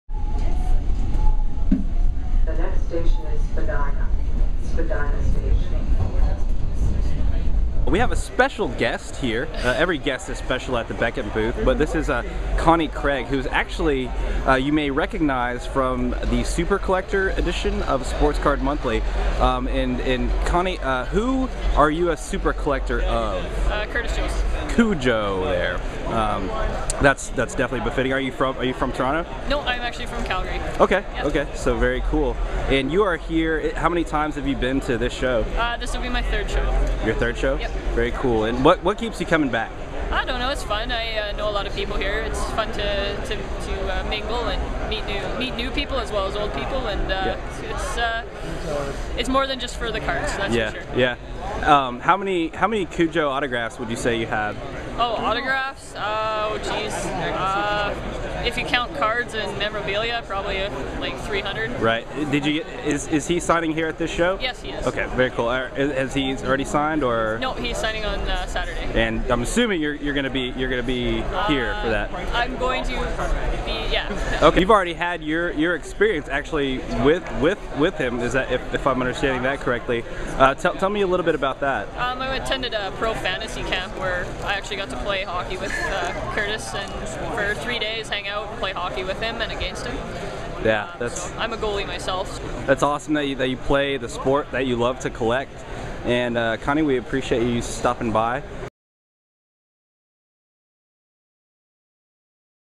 CUJO SuperCollector Sighting
Collector, goalie, and all-around standup Canadian Connie Craig came by the Beckett booth to see if we had the SuperCollector issue of Sport Card Monthly on hand. She was featured in the magazine as a Curtis Joseph SuperCollector.

Check out our interview to see just how much CuJo cred she has.